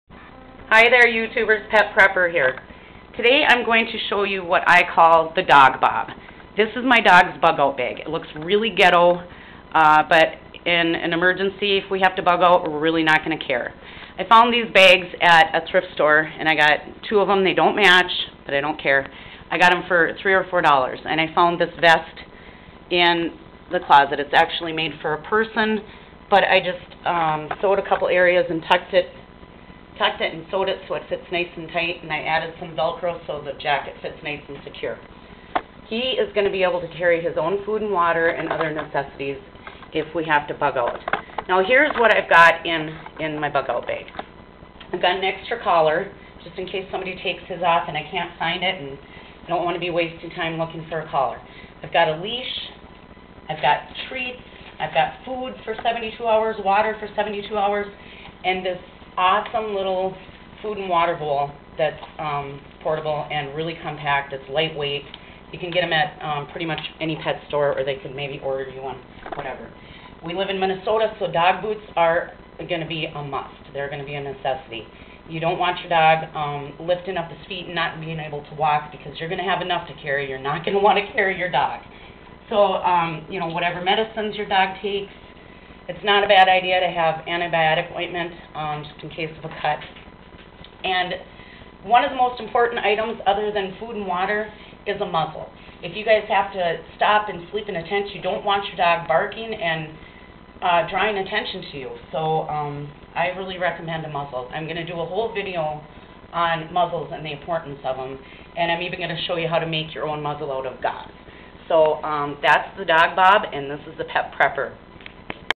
Dog back packs, make your own
I call this the DOGBOB.  It is my dogs Bug Out Bag.  I made it for around $4.  He hated it at first, but I am putting it on him daily, giving him treats and making a big deal out of it, he is starting to take to it.  And I have to admit, he looks pretty darn cute wearing it.
He looks like hes choking, but he stepped forward on the table just as I hit play.  Not every take can be perfect working with live animals.